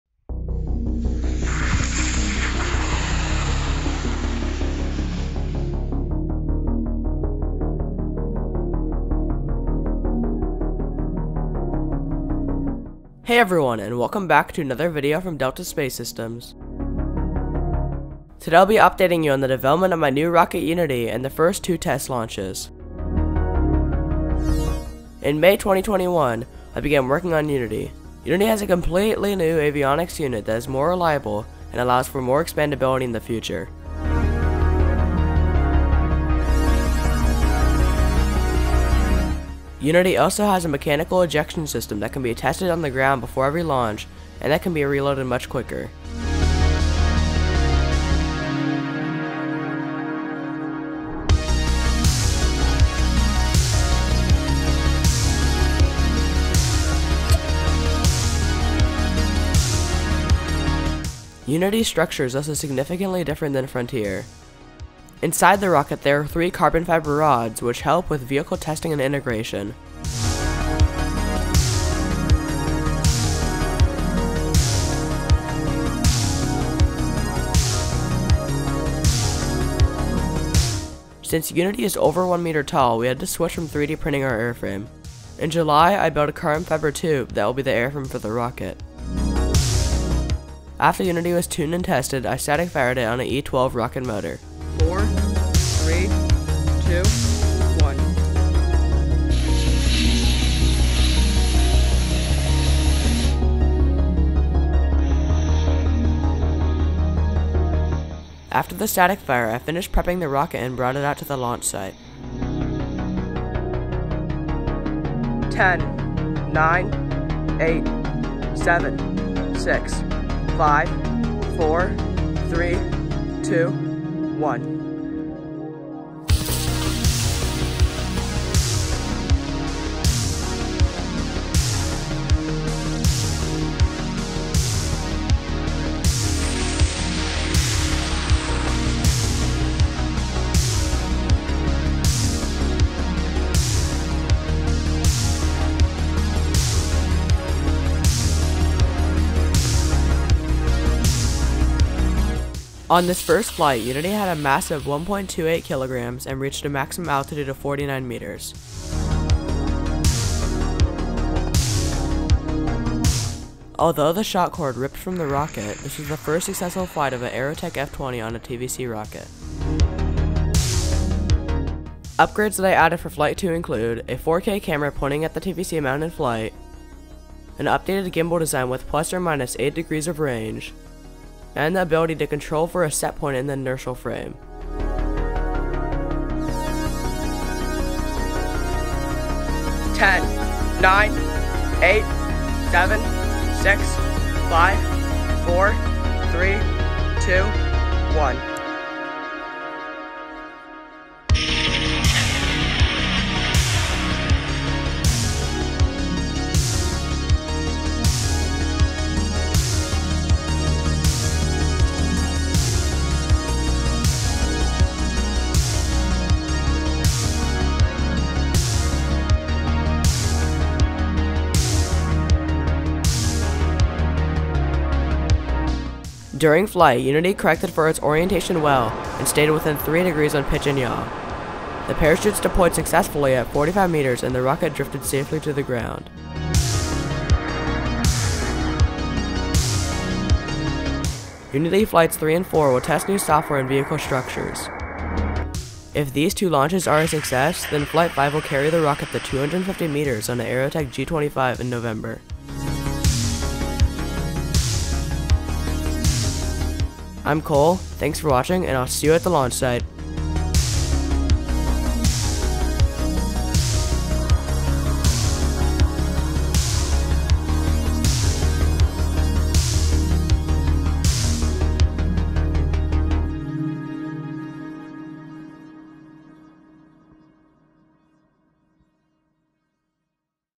Fully Successful TVC Flight | Flights 1 & 2
Hey Everyone! It has been a while since I have been testing and developing my newest rocket Unity! Unity improves on Frontier in many ways and it will be our workhorse in the future! More launches and development coming soon!

0:00 - Intro
0:23 - Development
1:35 - Static Fire
1:55 - Unity Flight 1
3:09 - Unity Flight 2